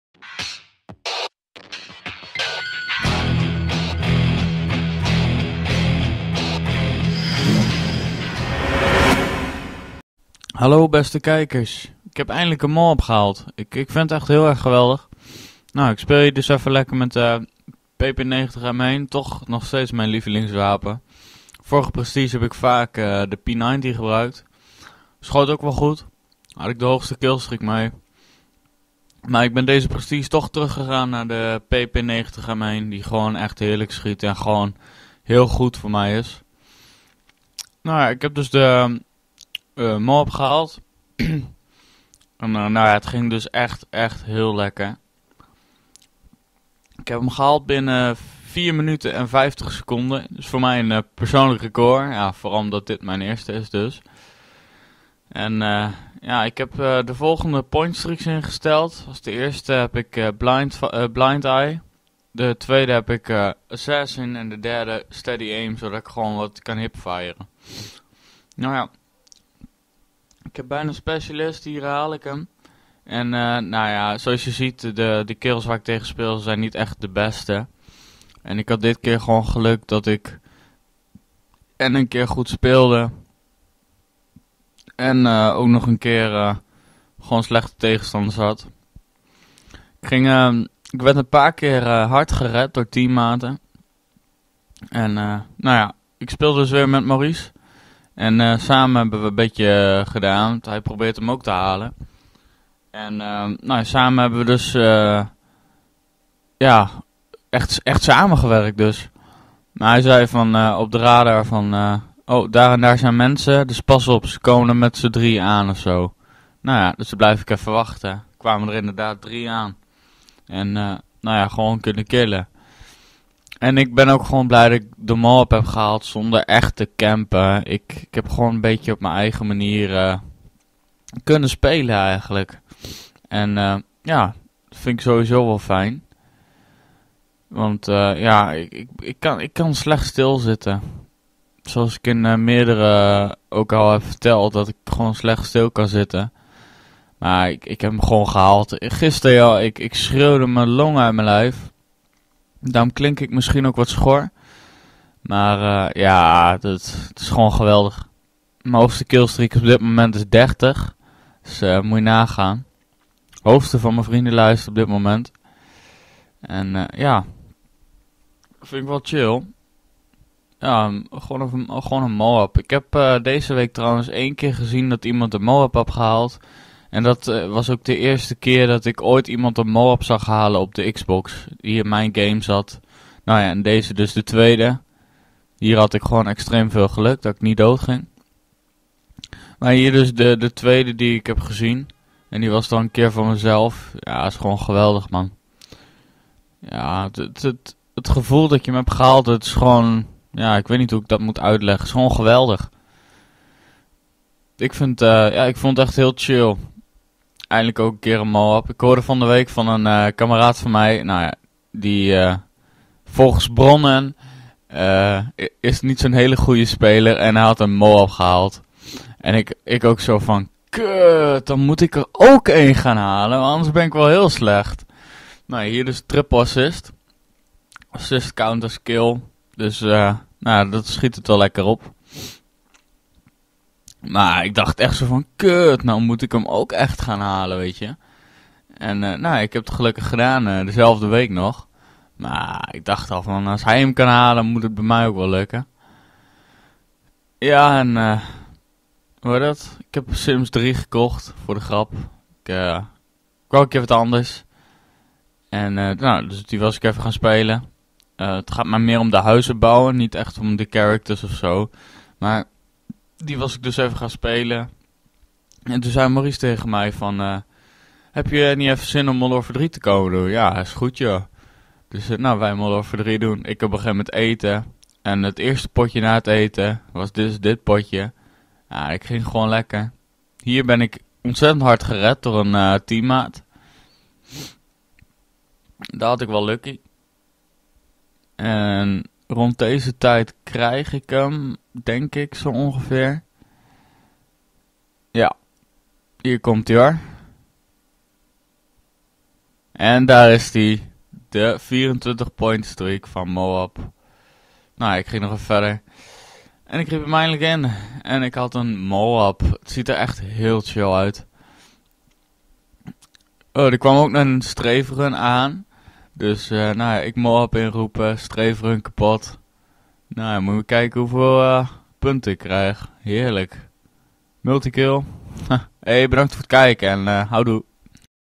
MOAB (Gameplay/Commentary)
Personal best MOAB time: 4 min 45 seconds.
First MOAB! :)